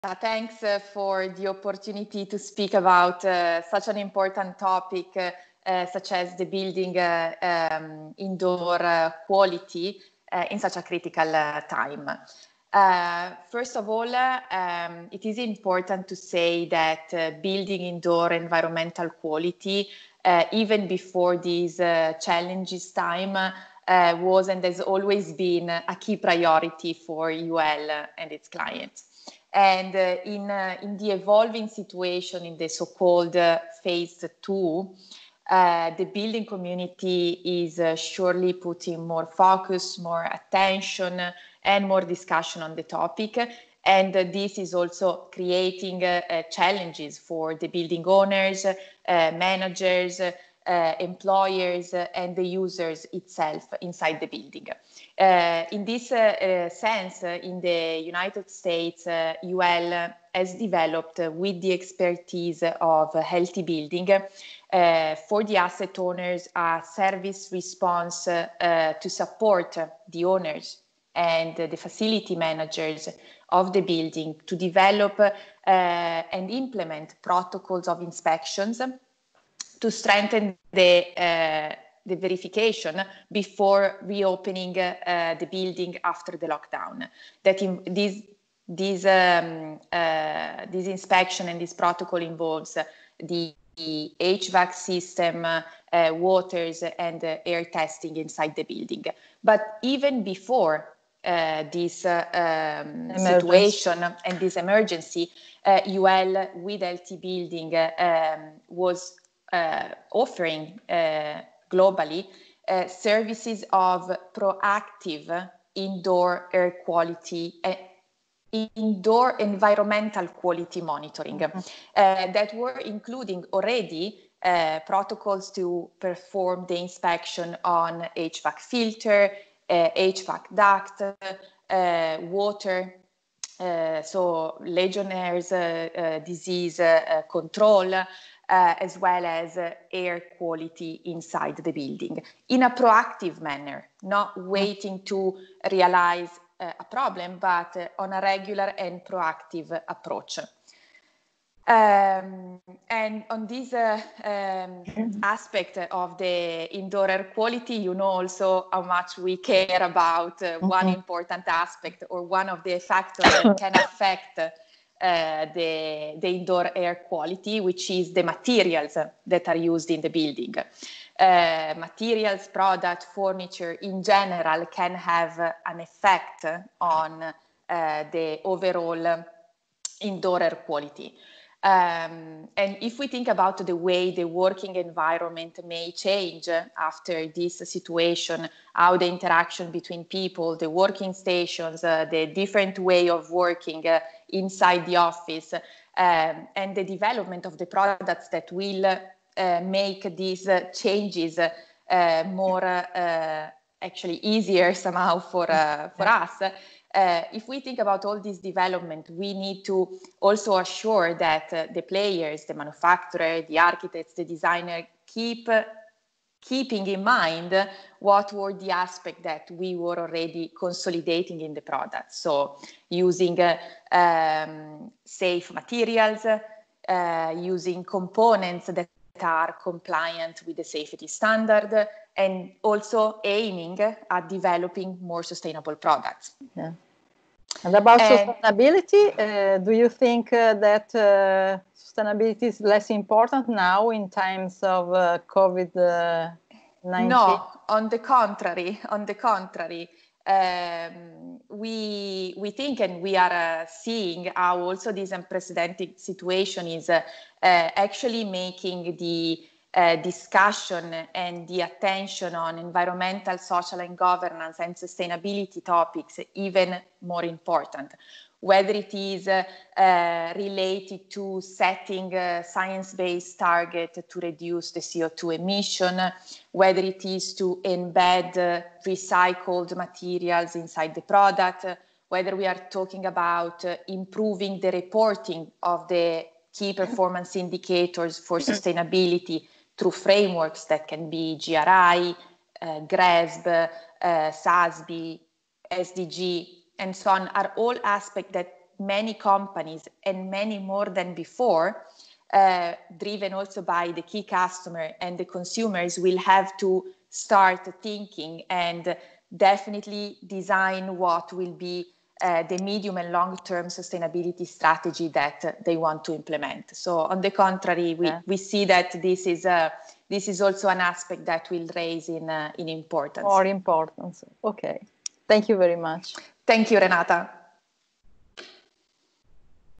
The indoor quality becomes increasingly important after covid-19 emergency.
Elena Veneziani, from UL's Environment and Sustainability Division explains in this video interview why the current health emergency makes the indoor quality increasingly important.
Actually, the aspects to be focused, the protocols and monitoring to be carried out for phase 2 and for the next phases confirm the validity of the approach Healthy Building and UlL have always supported even before the health emergency.